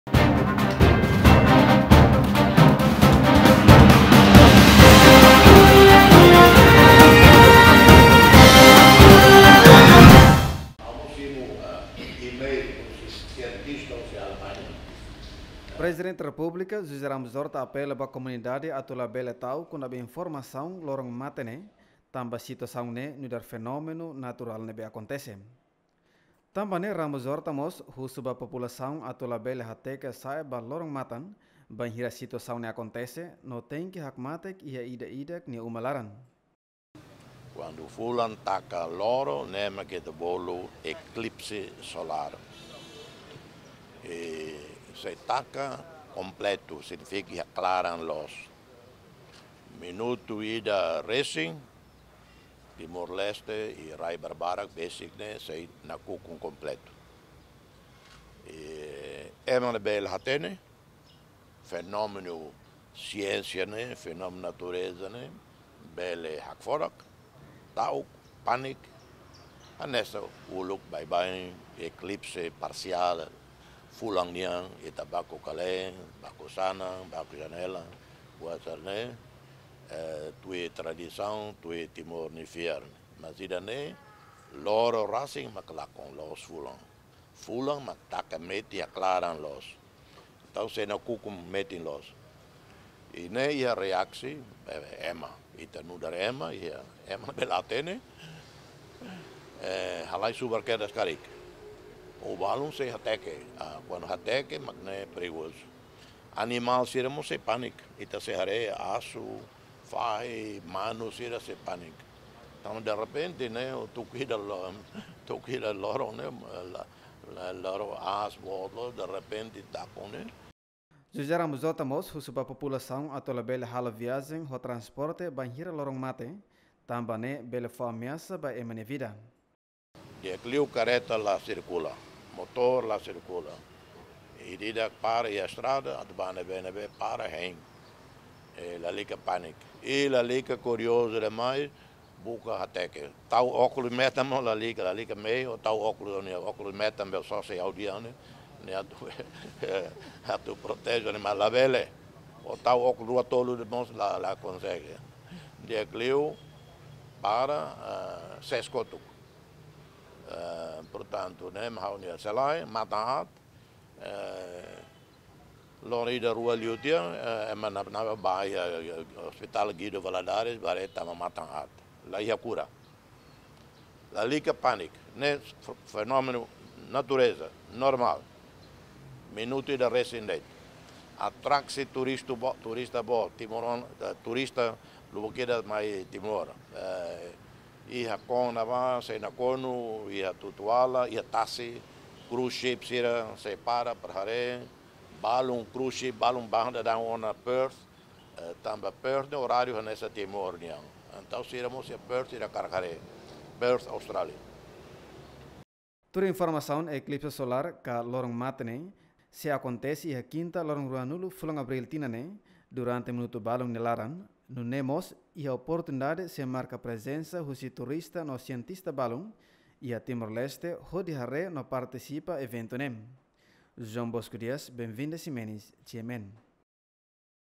Fenómena Eclipse Solár, PR apela ba Komunidade lalika koriozu atu buka hateke
Fenómena Eclipse Solár, PR apela ba Komunidade lalika pániku koriozu atu buka hateke